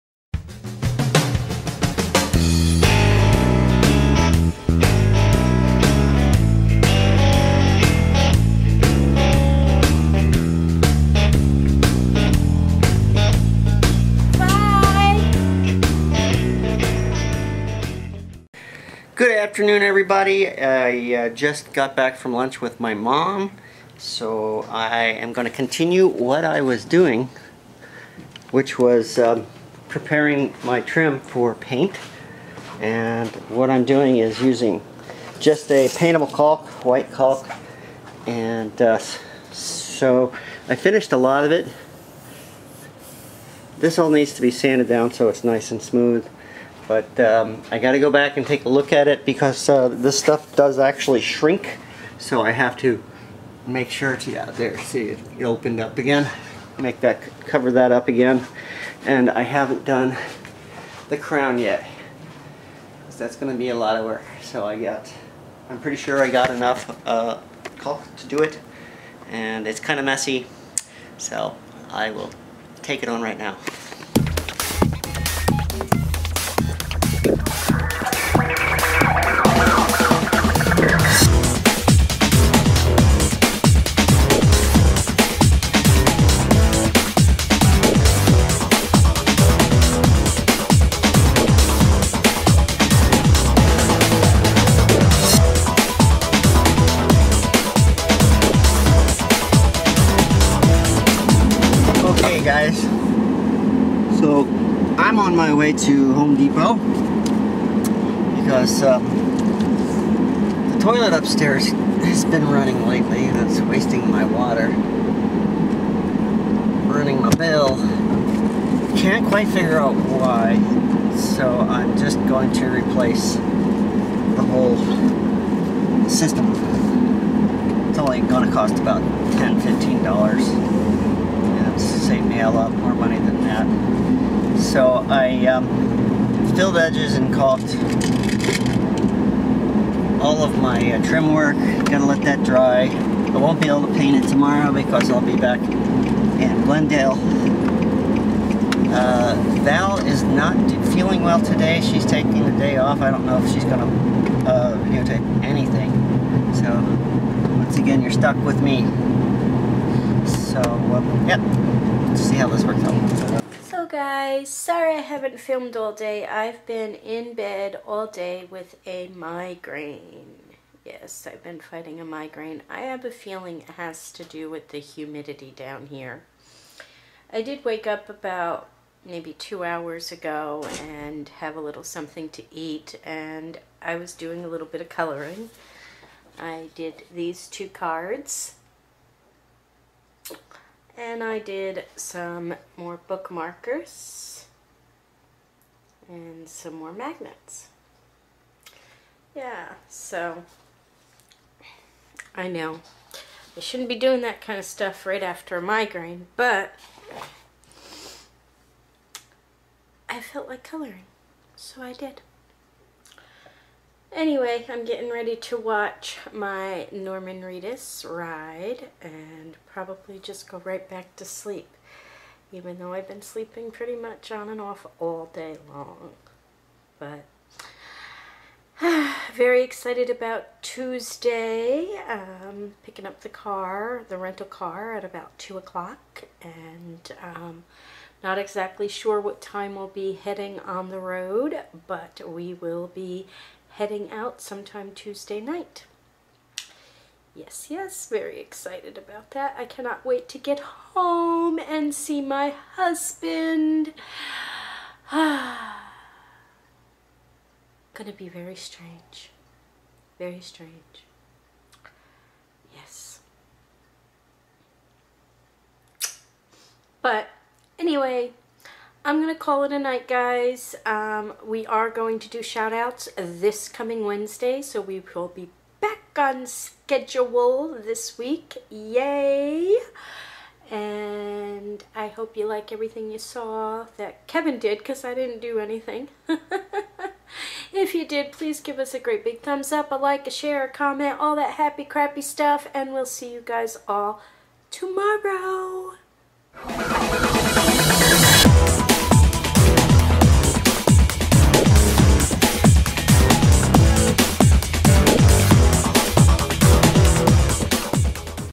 CALKING CROWN
Kevin does some more prep work in the Dining Room and Kitchen so he can finally finish the painting then deals with toilet troubles! Valerie fights with a migraine and then does some coloring!

                    Kevin: 49drummers
                    Megan: meganceciliacoyne